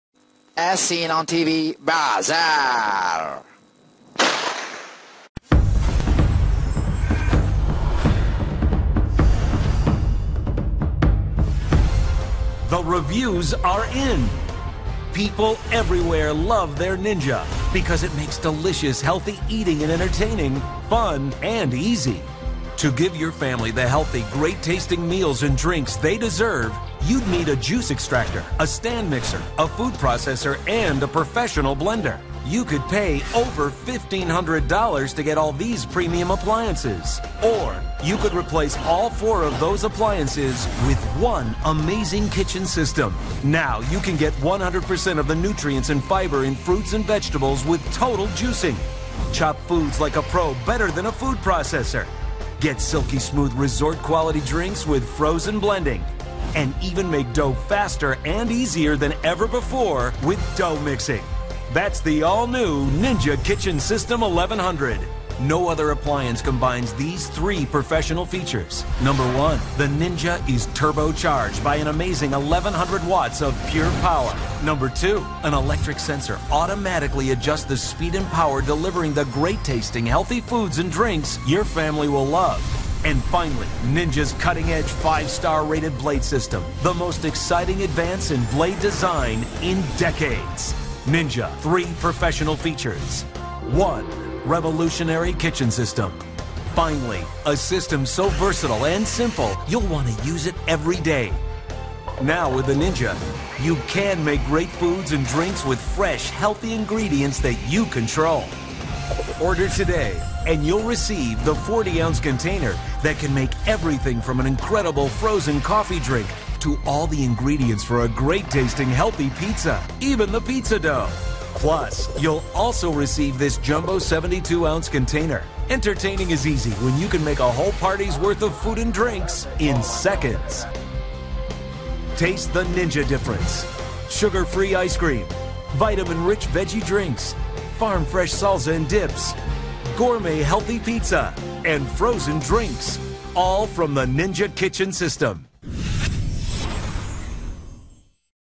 Ninja Kitchen System 1100 - As Seen On TV Ninja 1100 4 in 1 System
- Click the link for more details and info about the Ninja Kitchen System 1100.
Ninja Kitchen System 1100 is the ultimate 4in1 kitchen system. It is a Ninja Mixer, Ninja Blender, Ninja Juicer and a Ninja Food Processor. Learn more by visiting the links above.